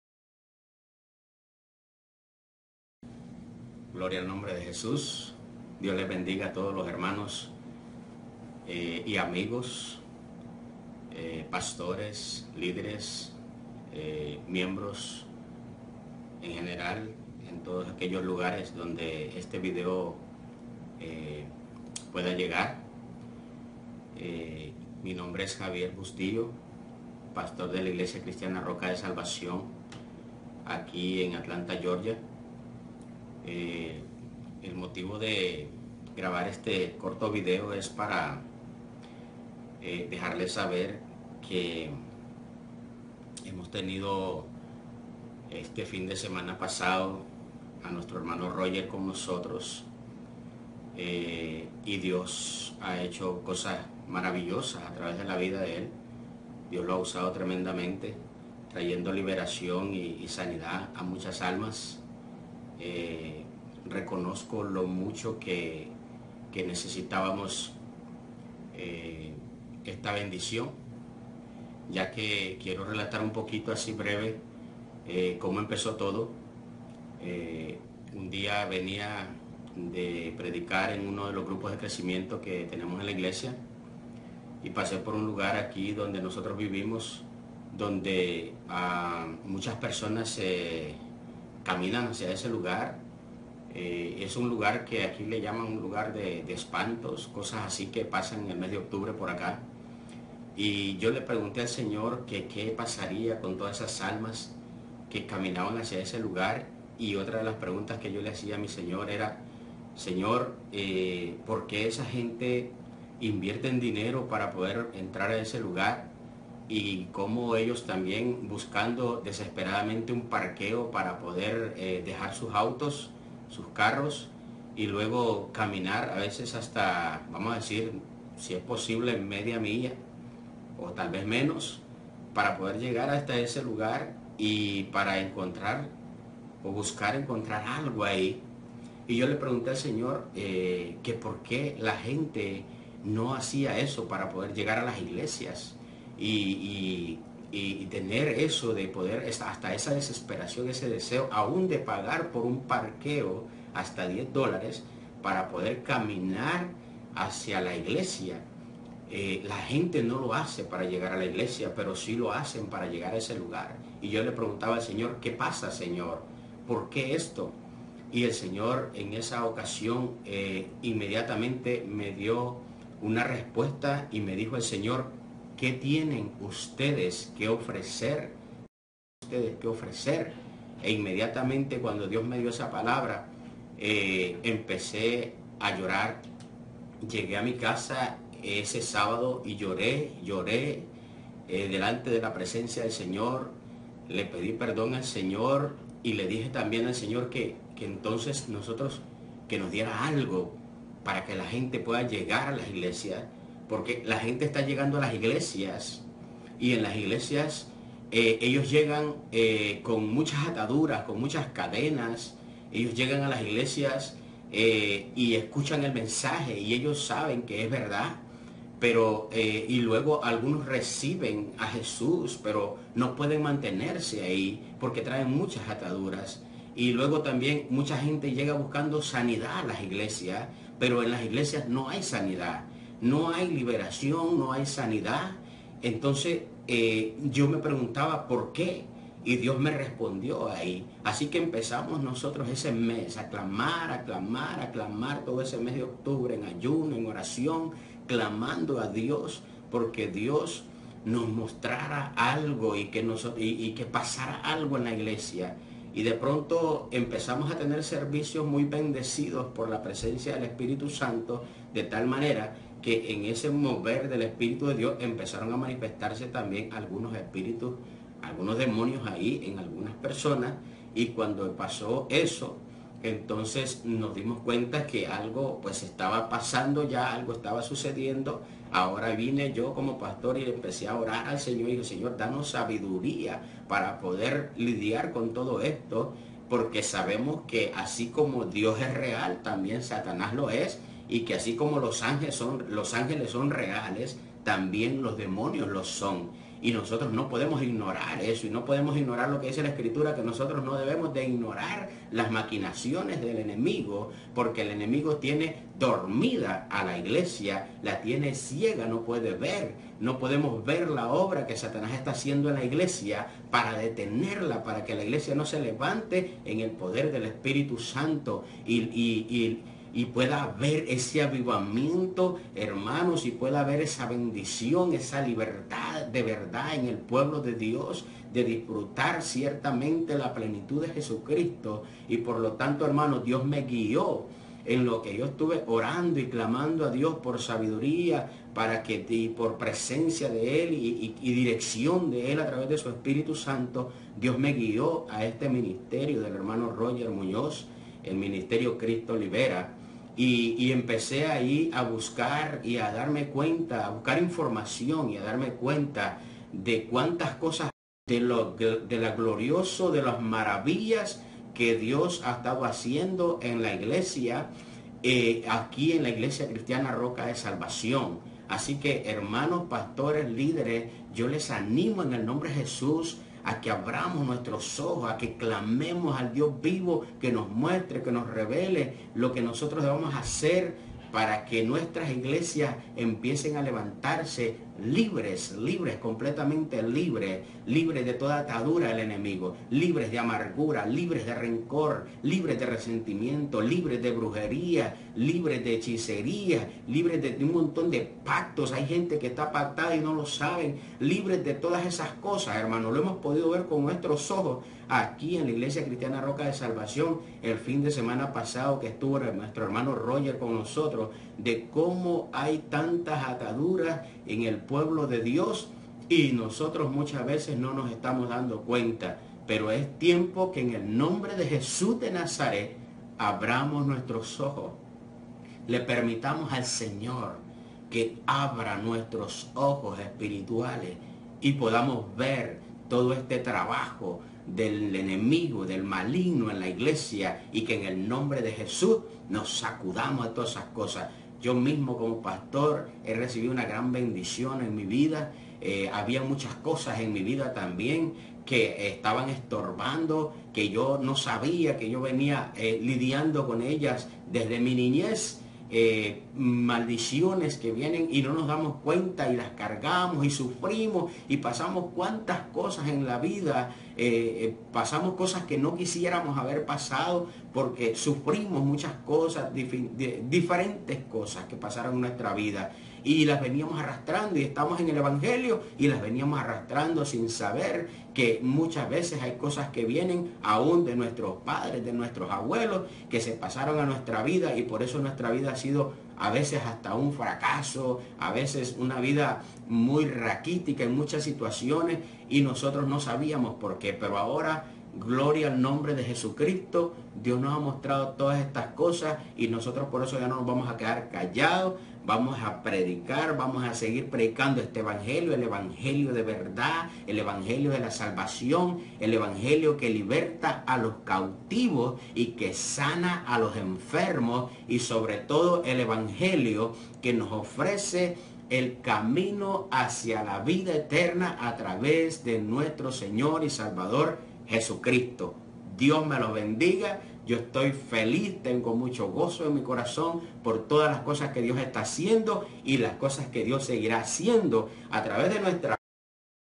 ¿Que tienen ustedes para ofrecer? me pregunto Jesus. Testimonio del Pastor Javier Bustillo.
Escuchen tremendo testimonio.
Pastor Javier Bustillo es el pastor de la iglesia Cristiana: Roca de Salvacion, en Giorgia, Atlanta.

AMAZON
APPLE STORE
BARNES & NOBLE
SCRIBD

SKYPE: ROGERNESTOR2003